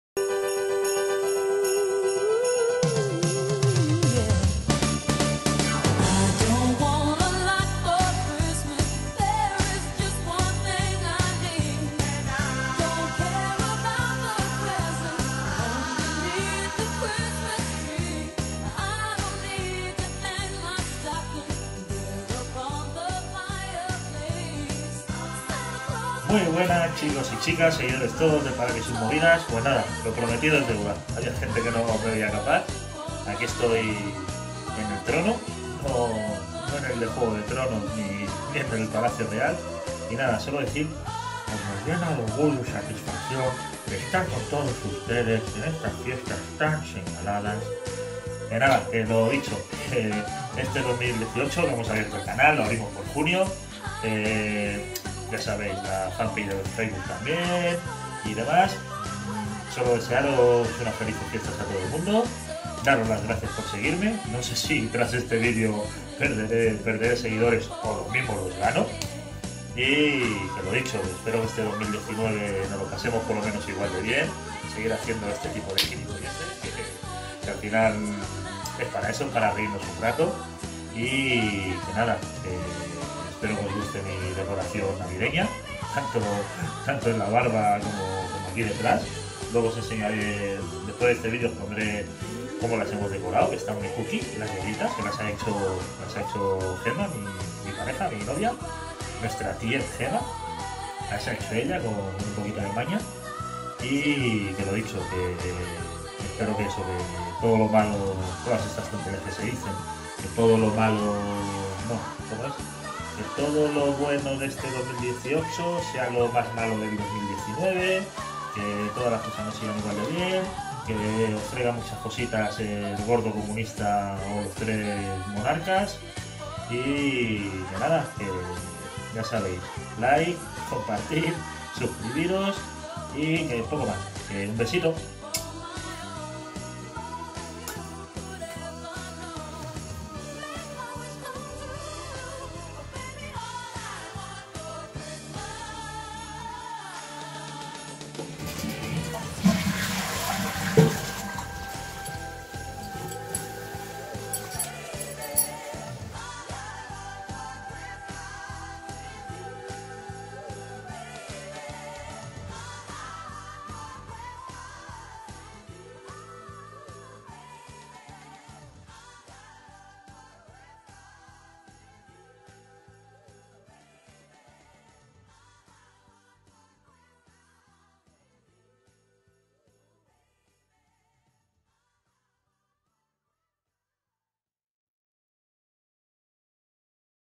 Felicitación navidades | Paraca felicitación | 2018 - 2019
Vídeo reto felicitando la navidad a seguidores y amigos al igual que a haters y enemigos.